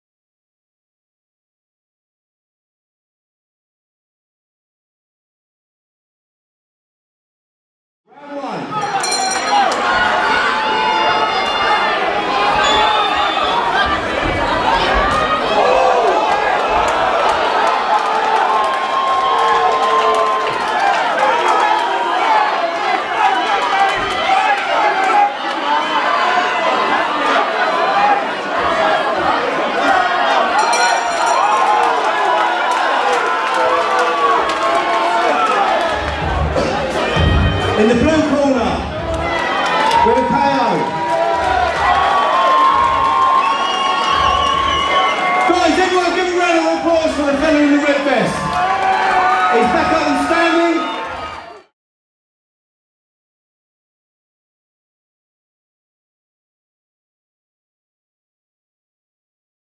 Telford | Ultra White Collar Boxing | Ross Oldcorn VS Matty Elsmore
Telford Ultra White Collar Boxing 30th March 2019

8 Weeks FREE training / Get in the best shape of your life/ Raise money for Cancer Research UK